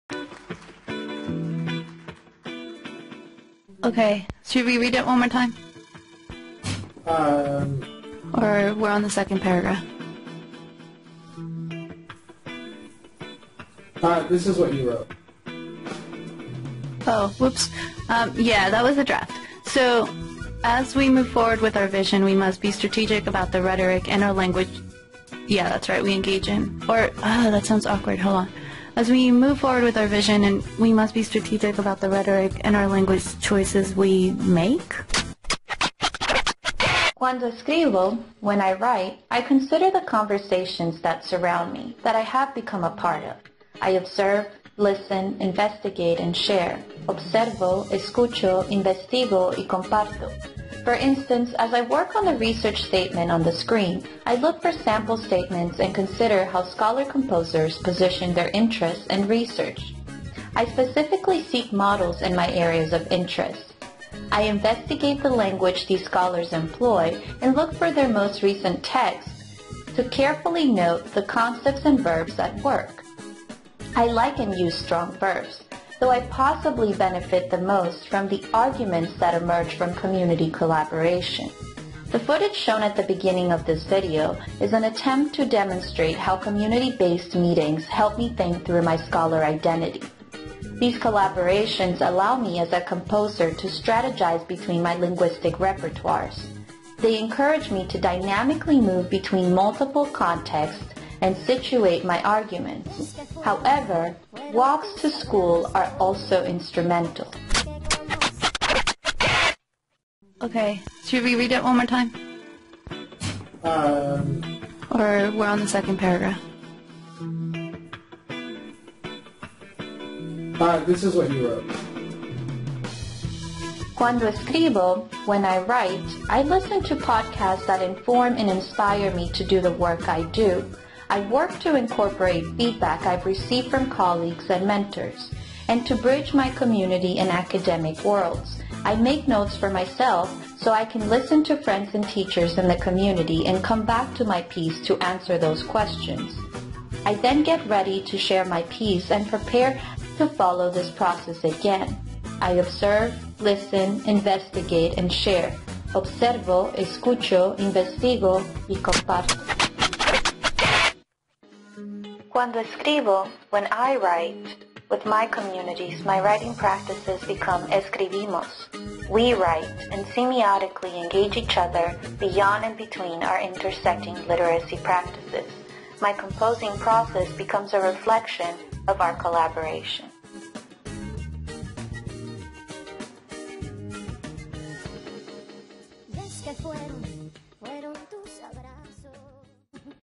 "'Cuando Escribo: When I Write'" by Sara P. Alvarez (Kairos 21.2 Praxis) (Kairos 21.2 Praxis)
What does composing look like in and across digital, networked spaces and the physical spaces our bodies inhabit as we compose? In this project, the authors take on this question and others as they capture and share their composing processes across mediums, platforms, localities, and languages. This text is published in the January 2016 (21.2) issue of the Praxis section of Kairos: A Journal of Rhetoric, Technology, and Pedagogy. Kairos is a free, online, peer-reviewed scholarly journal available